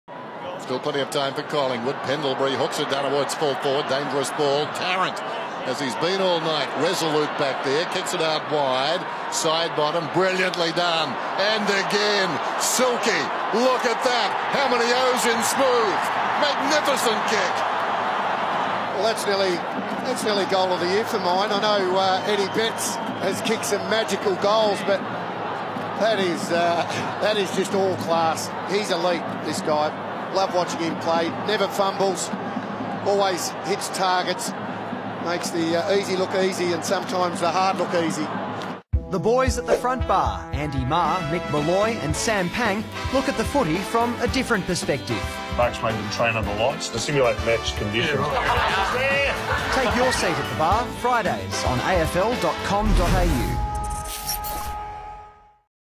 Sidebottom's great finish - AFL
Steele Sidebottom kicked a beauty against the Kangaroos.